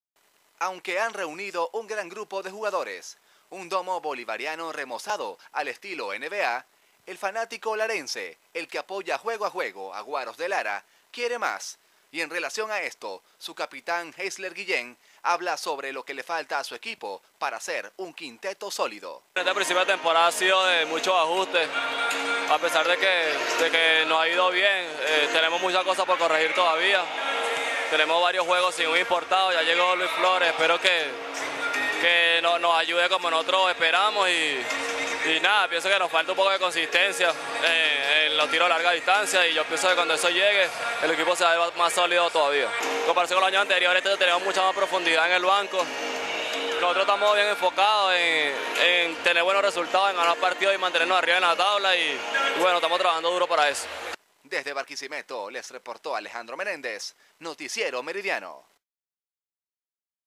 Entrevista con Heissler Guillent Guaros de Lara
El Capitán de Guaros de Lara, Heissler Guillent, habla sobre la actualidad de su equipo